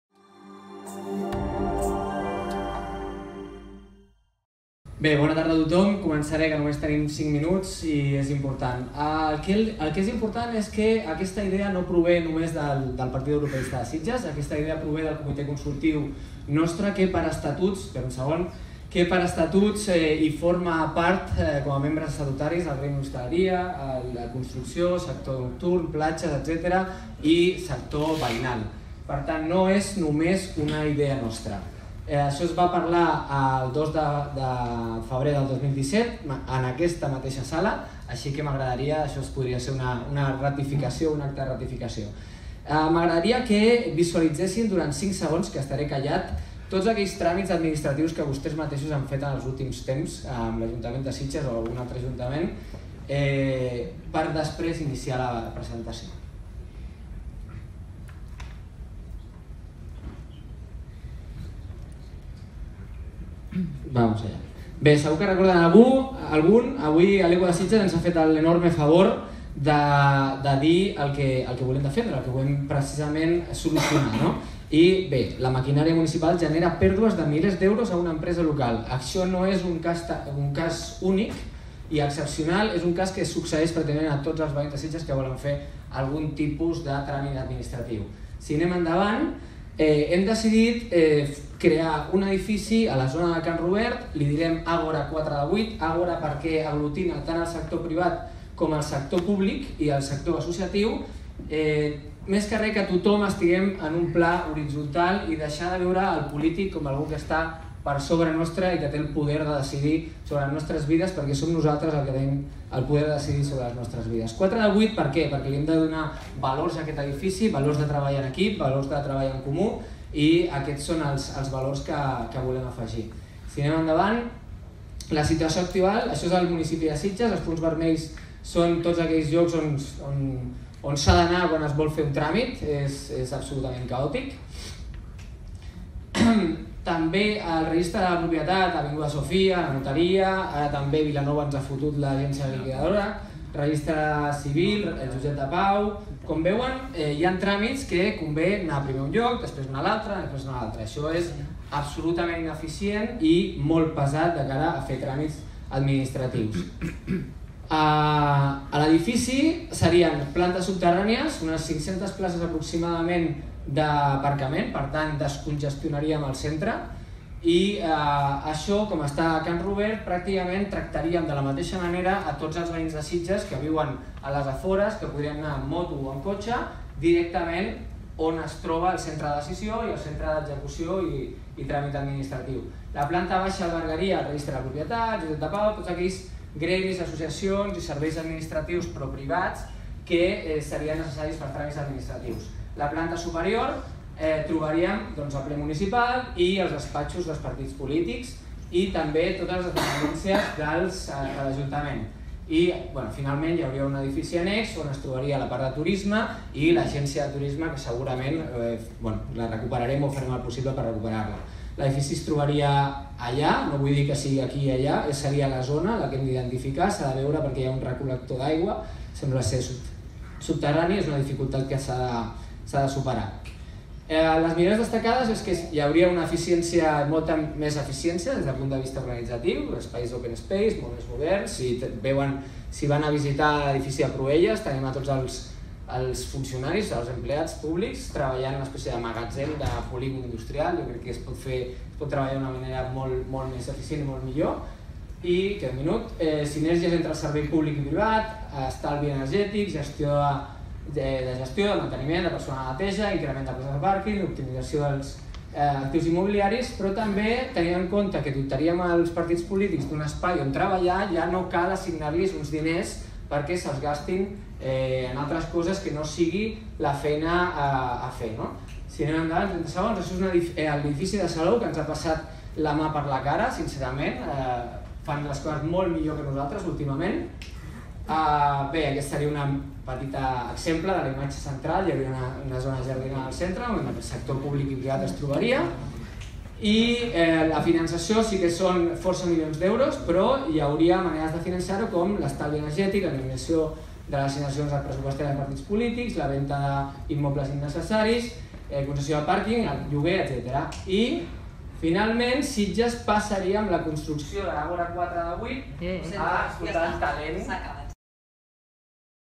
El Partit Europeïsta de Sitges pretén un gran edifici per centralitzar l'Ajuntament
El cap de llista del Partit Europeïsta de Sitges, Francesc Alcaraz, vol centralitzar els serveis administratius de l'Ajuntament en un gran edifici. Ho ha explicat a la jornada Repensar Sitges, organitzat per L'ECO de Sitges.